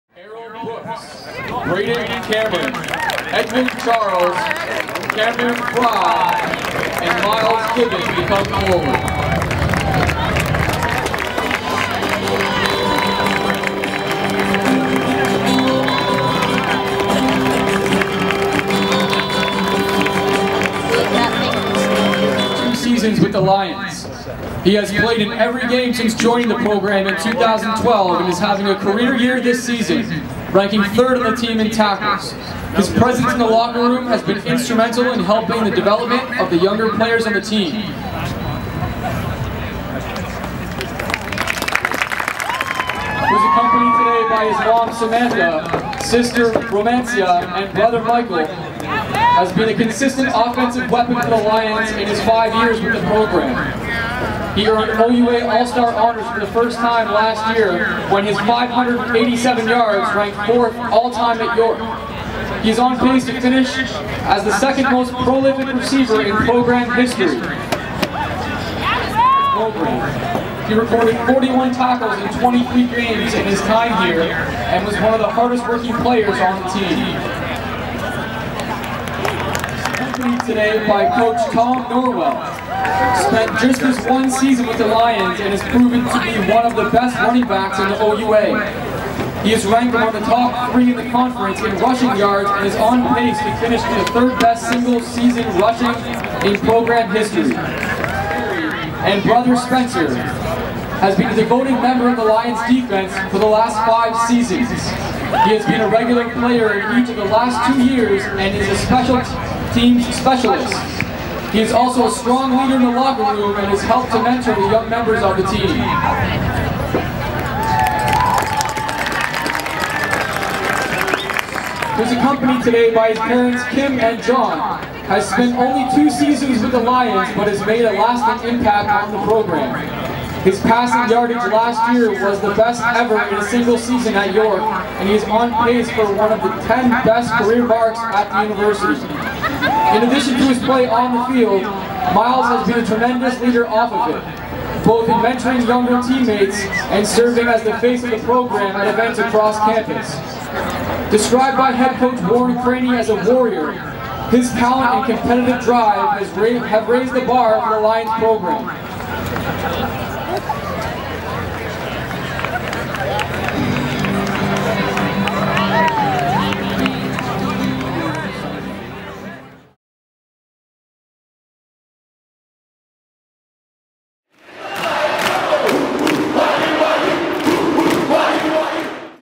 York Lions | Graduating football players honoured in final home game - October 10, 2013
YORK HONOURS SIX SENIORS AT FINAL HOME GAME

York University Sport & Recreation honoured its six members of the football team that played in their final varsity home game before Thursday's contest against the Toronto Varsity Blues.